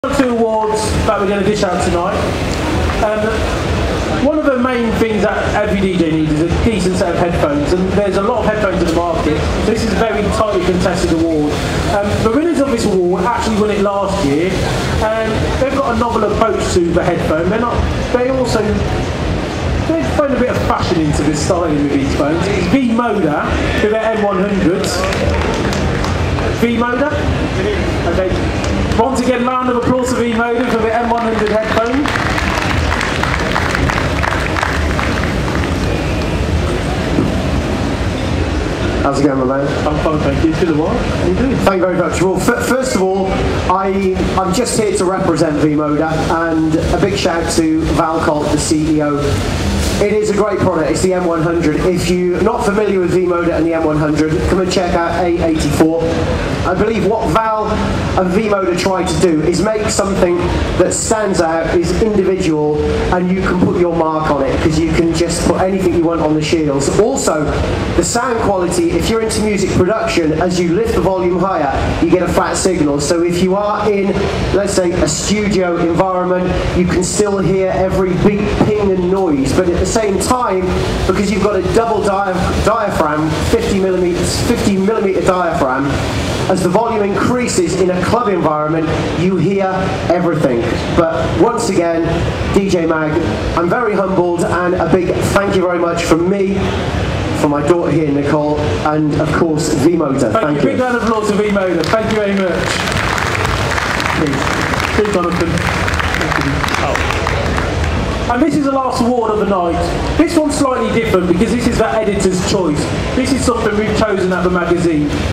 V-Moda Crossfade M-100 Headphones win DJ Tech Award @ BPM Show 2014
V-Moda Crossfade M-100 Headphones win DJ Tech Award 2 years running for 'Best Headphones'.